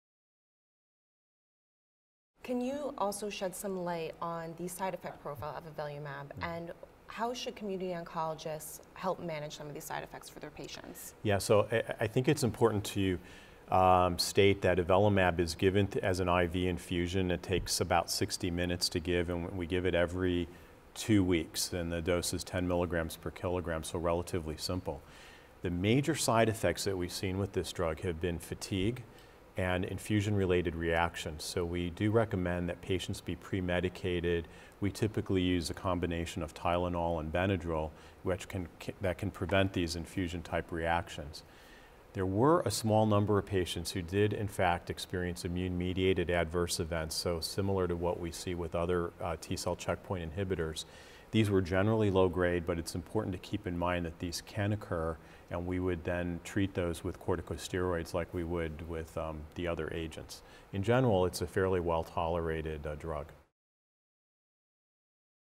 Managing Avelumab Side Effects in Patients With Merkel Cell Carcinoma
Howard L. Kaufman, MD, FACS, chief surgical officer and associate director for clinical science at Rutgers Cancer Institute of New Jersey, discusses the adverse event profile of avelumab (Bavencio) for patients with Merkel cell carcinoma.